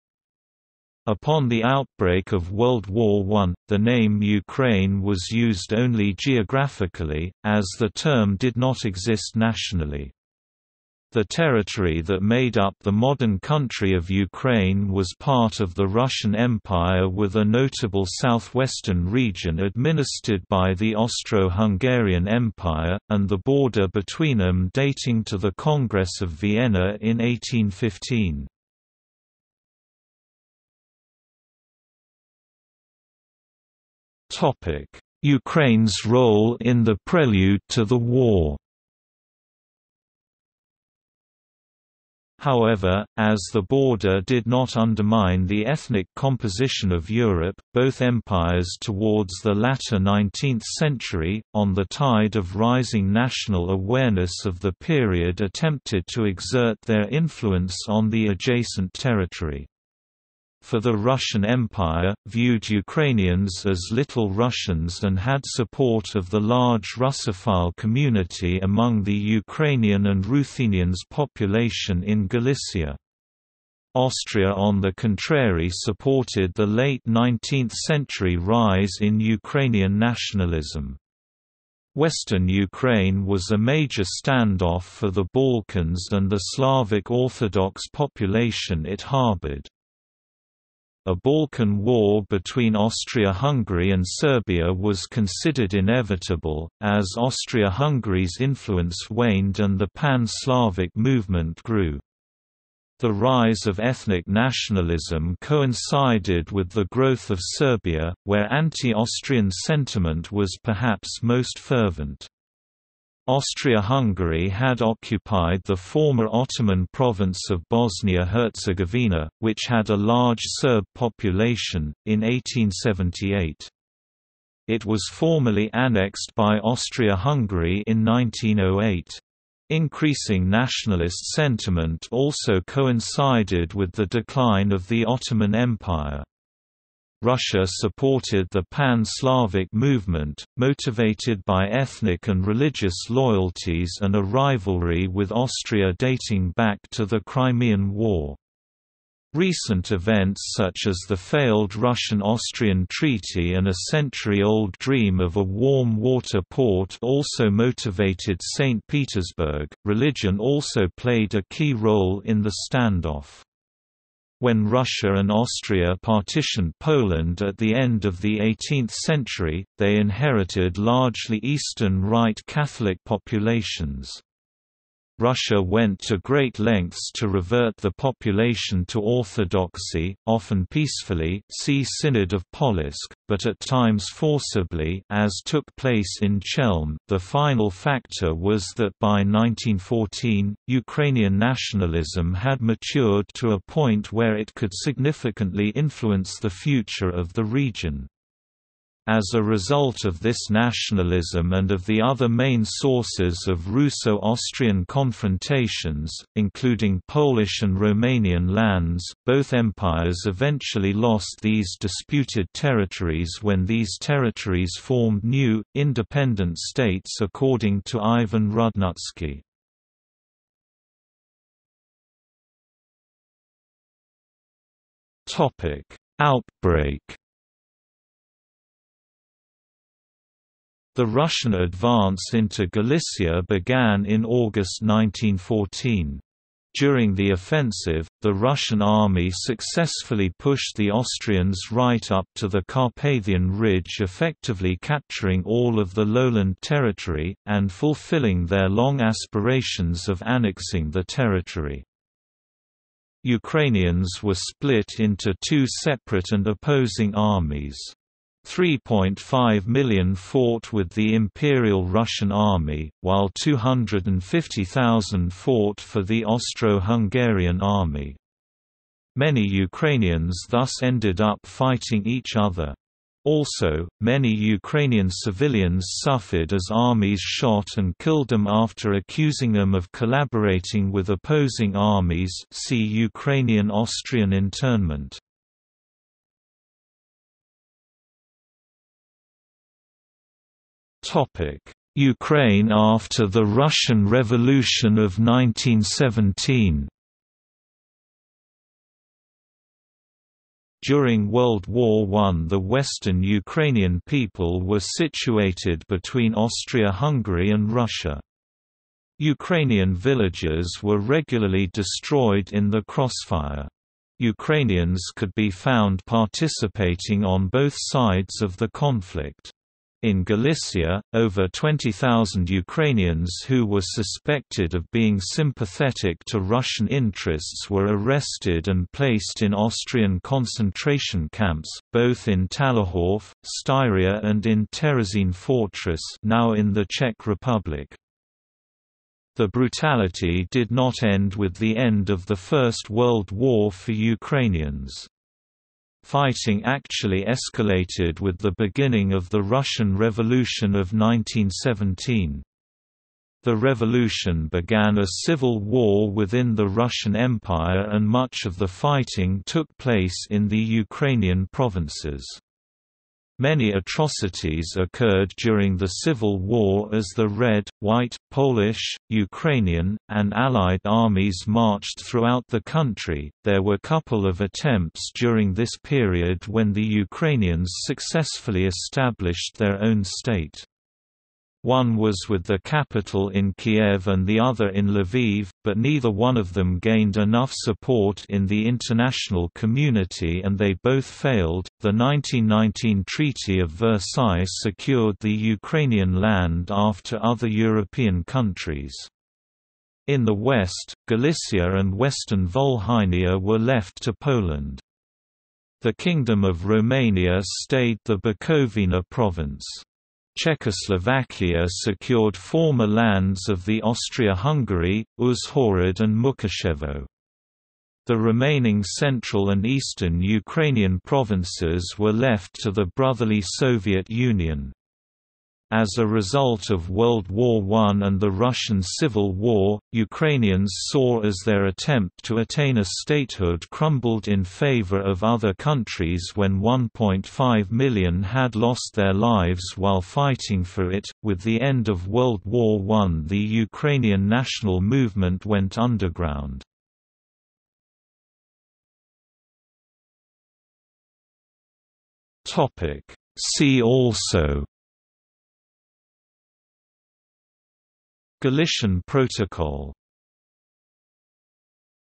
Ukraine during World War I | Wikipedia audio article
00:00:30 1 Ukraine's role in the prelude to the war
00:03:41 2 Outbreak
00:04:47 3 Ukraine after the Russian Revolution of 1917
00:07:28 4 See also

- Socrates

SUMMARY
Upon the outbreak of World War I, the name Ukraine was used only geographically, as the term did not exist nationally. The territory that made up the modern country of Ukraine was part of the Russian Empire with a notable southwestern region administered by the Austro-Hungarian Empire, and the border between them dating to the Congress of Vienna in 1815.